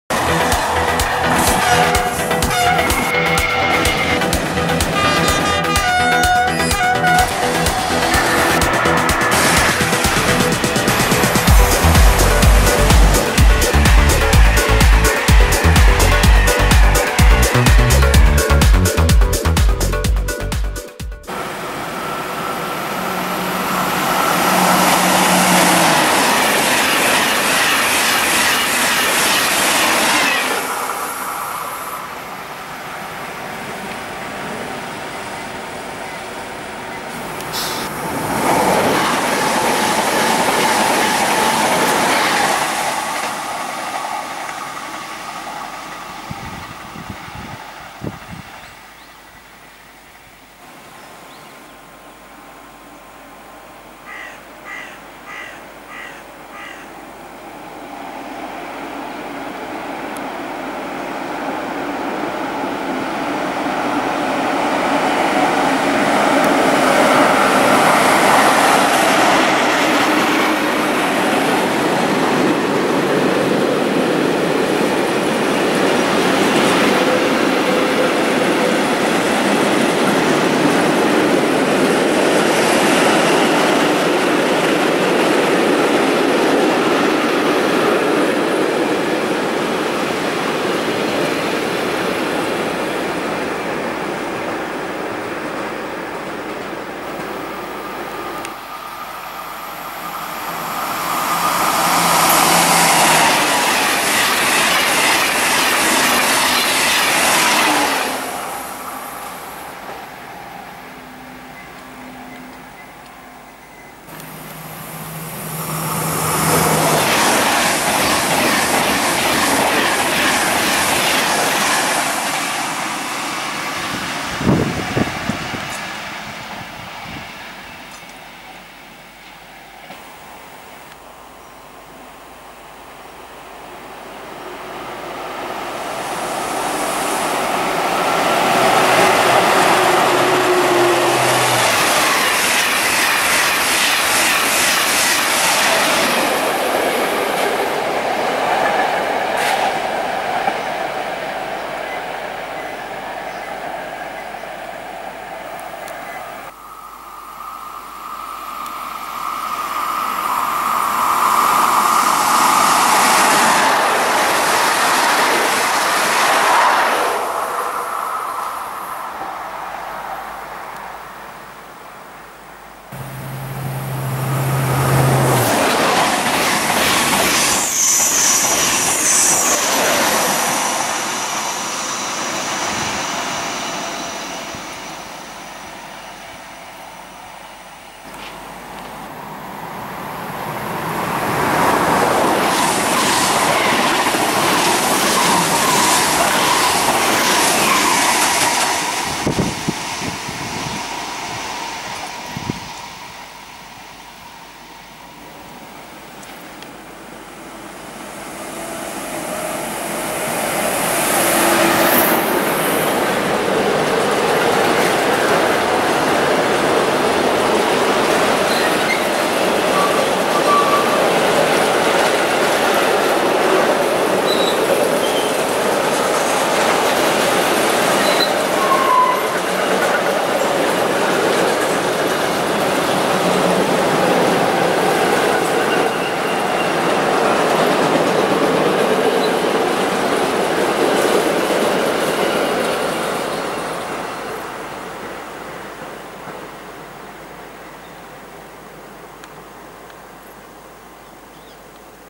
Gare de Le Chénay - Gagny #8: CC 40110 du MFPN et le "Champagne Express" (Spécial, TER, TGV)
Le MFPN avait fait circuler ce jour un train spécial, la "Champagne Express", composé de voitures historiques remorquées par la CC 40110, locomotive construite en 1970 classée Monument Historique, au départ de Paris et à destination de Reims via Épernay. Une correspondance en autorail était assurée à Reims pour rejoindre Givet.
Les CC 40100 furent missionnées sur Paris - Bruxelles avec les CC 18 de la SNCB.

00:00 Intro
00:22 Trafic de l'axe Paris - Strasbourg
02:11 CC 40110 et BB 69432 avec train touristique du MFPN (Désolé pour le Ouigo!)
02:36 Trafic de l'axe Paris - Strasbourg - Fin et liens